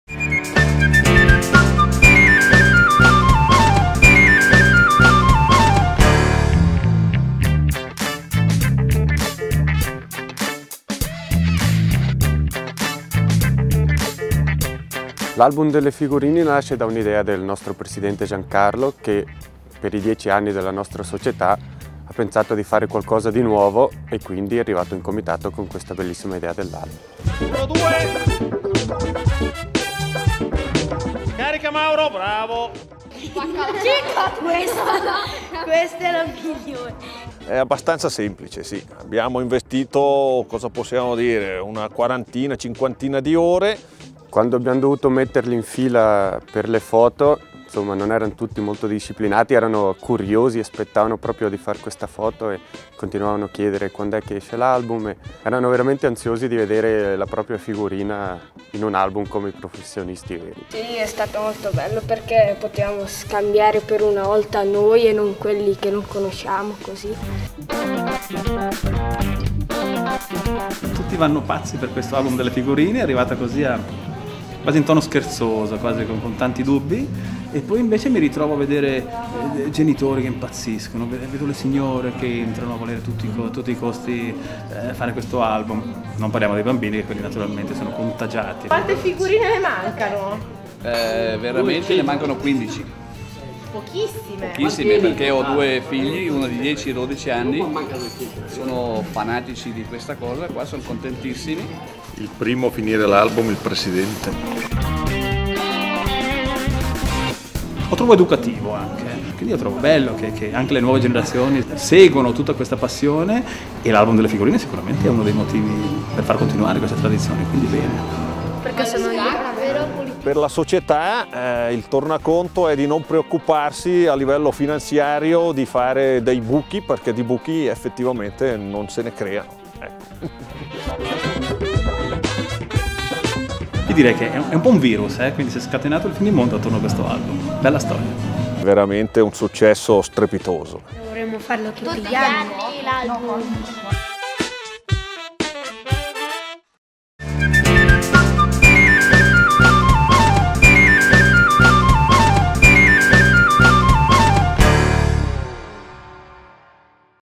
Valposchiavo - album figurine Valposchiavo
Guarda come la squadra di Calcio Valposchiavo ha realizzato il suo personale album di figurine con Akinda. Un'iniziativa che entusiasma tutti, dai ragazzi che si sentono come dei piccoli campioni, ai genitori che ricordano i loro album delle figurine. Un successo strepitoso da ripetere tutti gli anni! Zero costi, zero preoccupazioni, guadagno garantito!